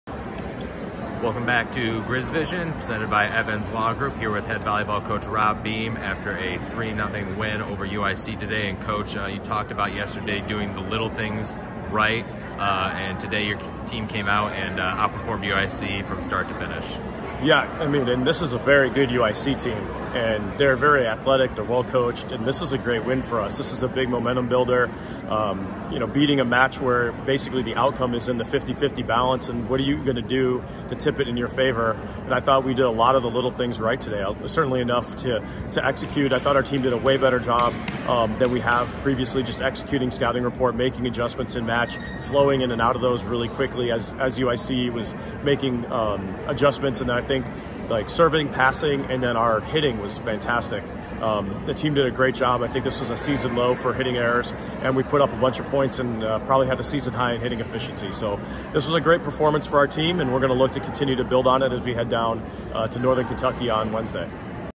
GrizzVision with Rob Beam 10/5/19
OaklandVB Head Coach Rob Beam talks about his team's 3-0 win over UIC.